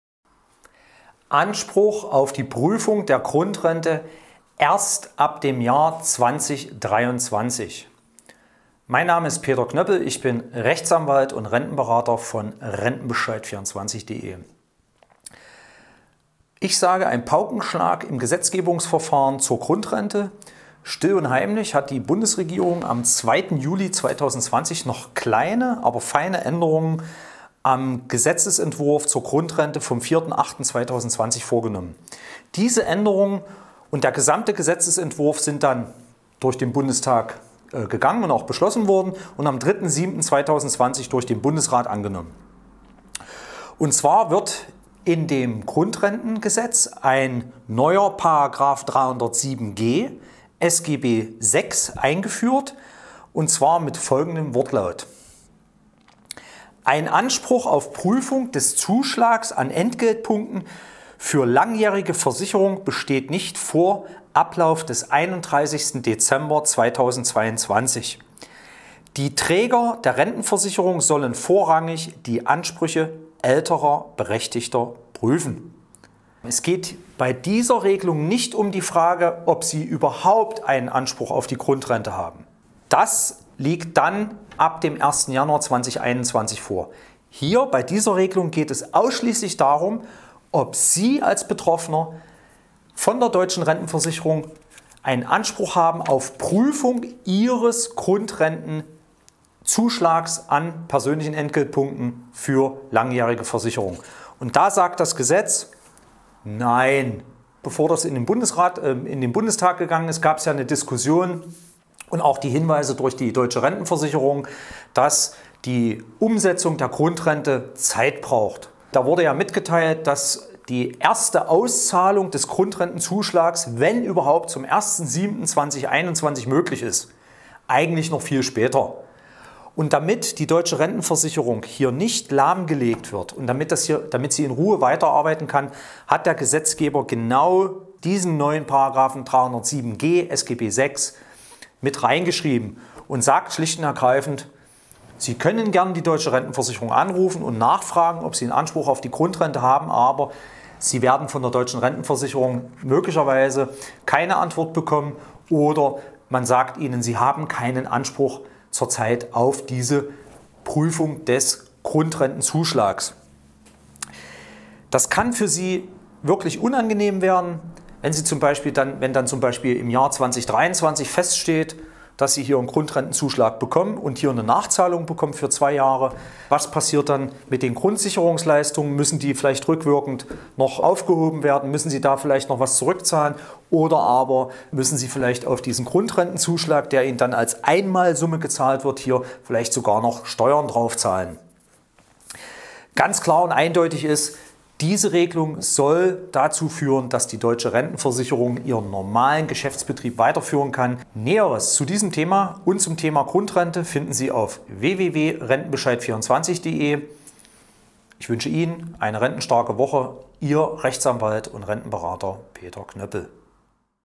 Grundrente § 307g - Anspruch auf Prüfung erst ab 2023
Wussten Sie schon: Die Deutsche Rentenversicherung bietet keine Beratung zum Thema Grundrente an und was genauso wichtig ist: Diejenigen Rentnerinnen und Rentner, die ab 2021 Anspruch auf eine Grundrente haben könnten, haben bis zum Jahr 2023 keinen Anspruch auf Prüfung der Grundrente. So sieht es eine kleine, aber wichtige Änderung im Grundrentengesetz vor.